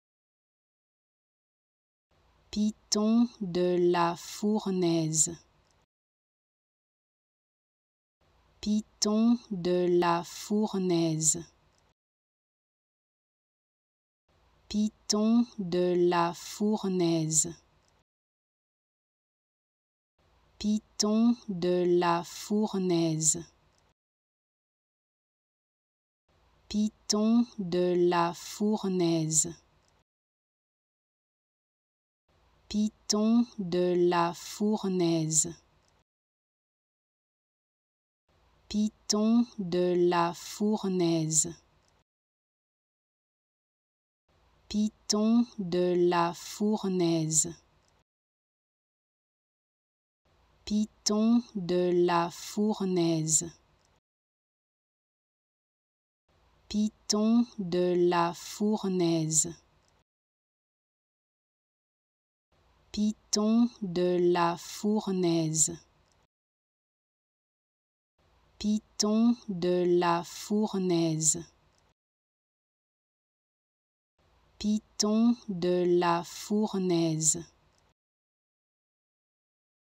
How to Pronounce ''Piton de la Fournaise'' Correctly in French
Learn how to say and properly pronounce ''Piton de la Fournaise'' in French with this free pronunciation tutorial. Apprenez à prononcer des mots en français grâce à nos tutoriels.